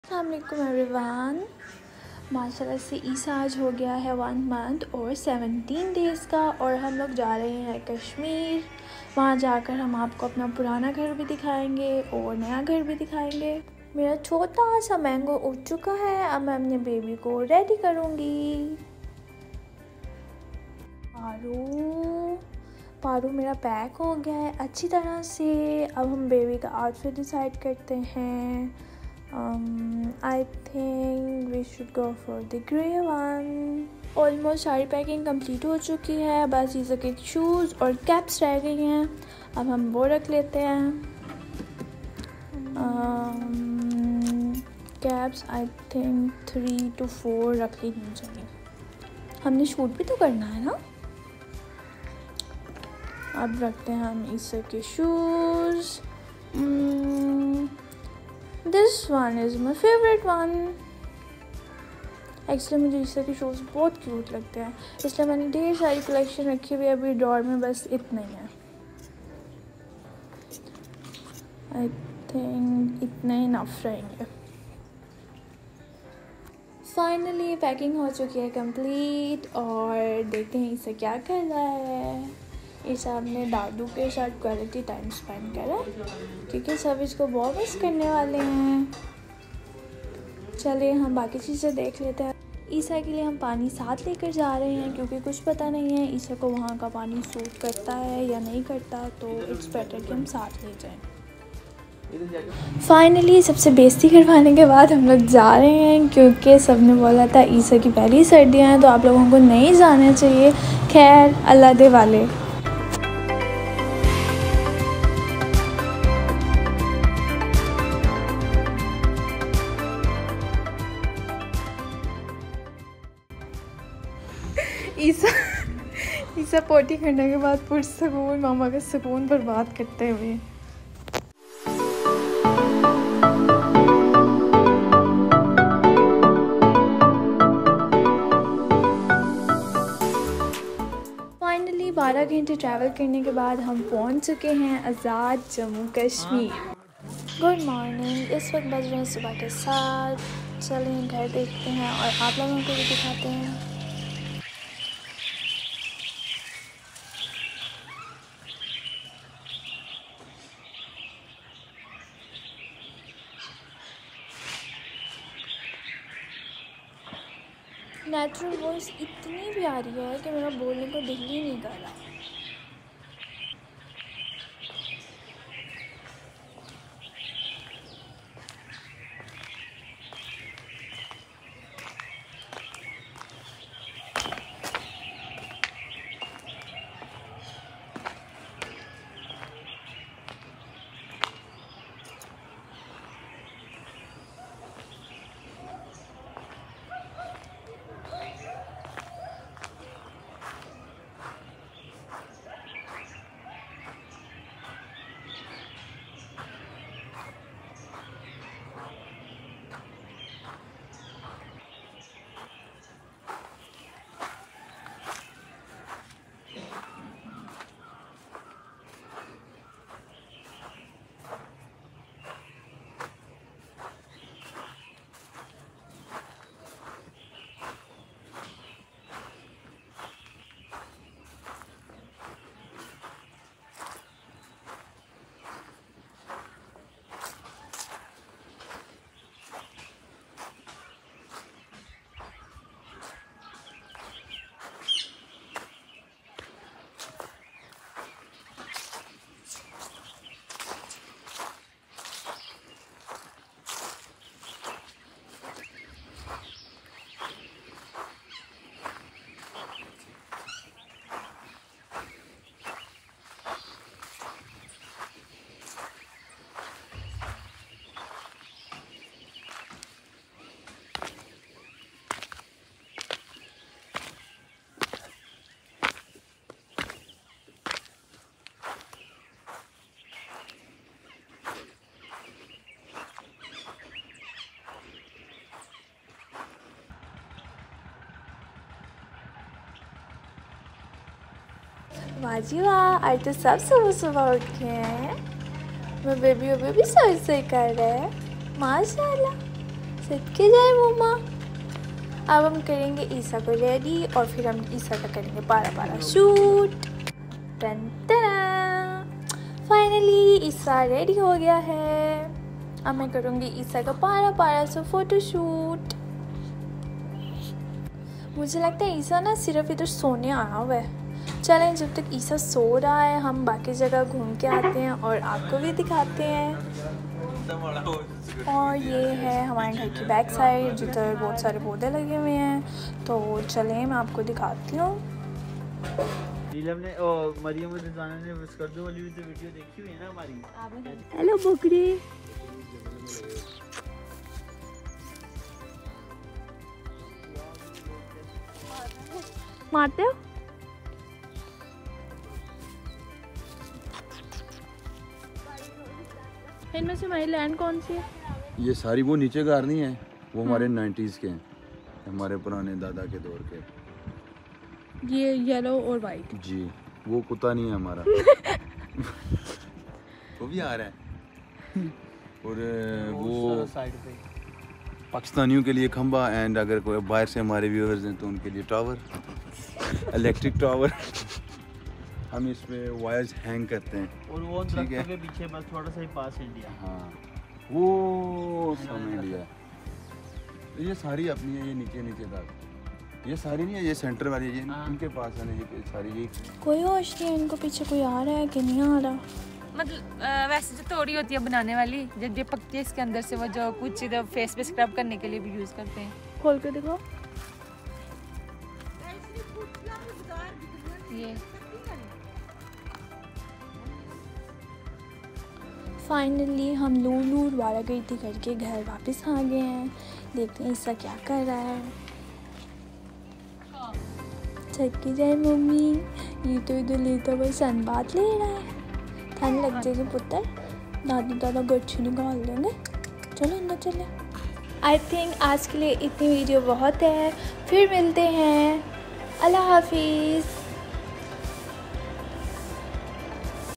Essa ka pehla trip| Azad jammu kashmir
Essa’s first trip to kashmir when he was just one month old